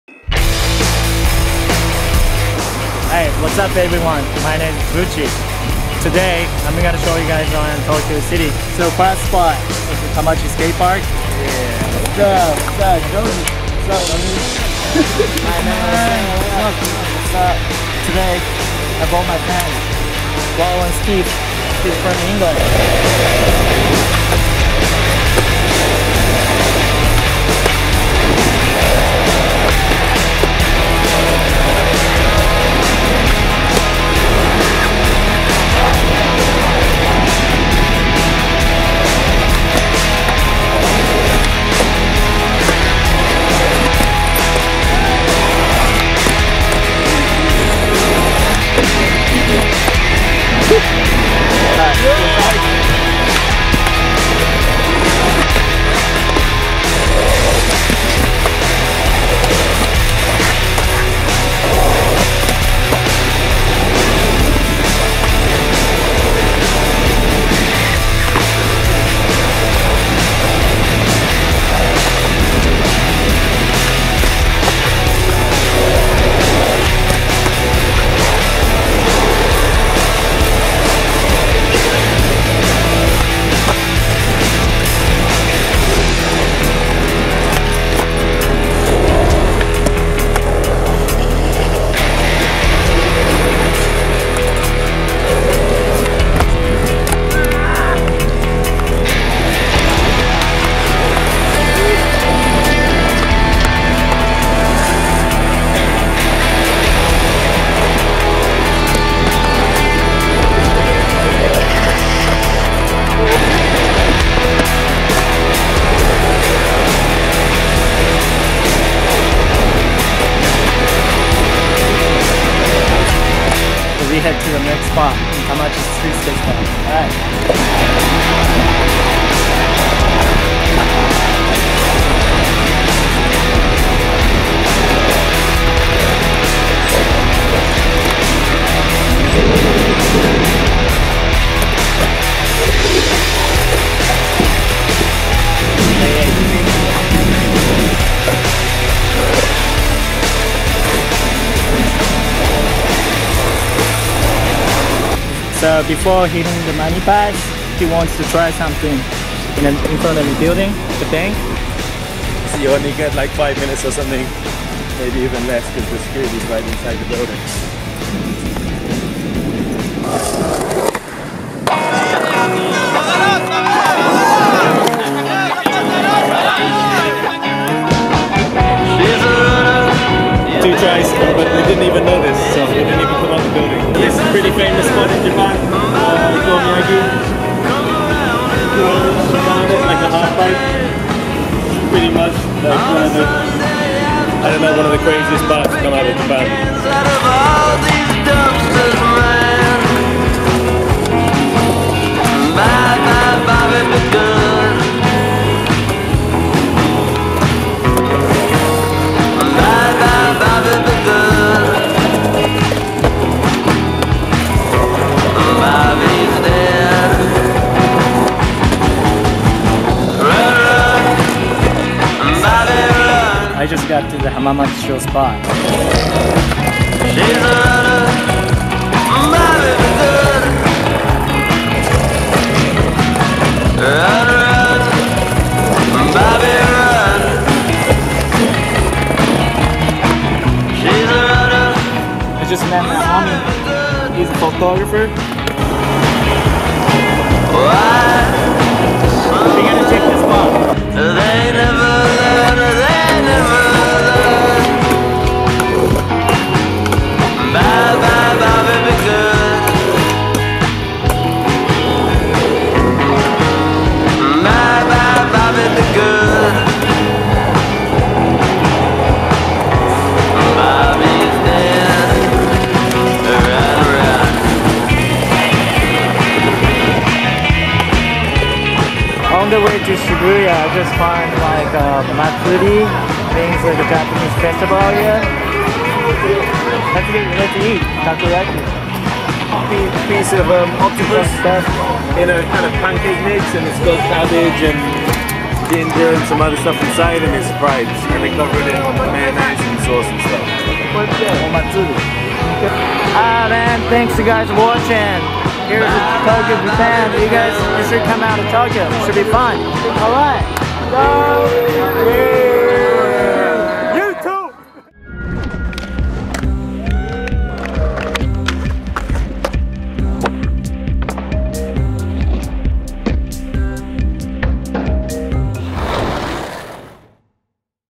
Skating in Tokyo, Japan with Hirotoshi Kawabuchi, Laurence Keefe & More!
Film/Edit:
Tomohiko Sumi @tomothehomeless

Skater:
Hirotoshi Kawabuchi @buchitokyo
Laurence Keefe @laurencekeefe
Naohiro Abe @naohiroabe
Joji Shimamoto @jojiphoto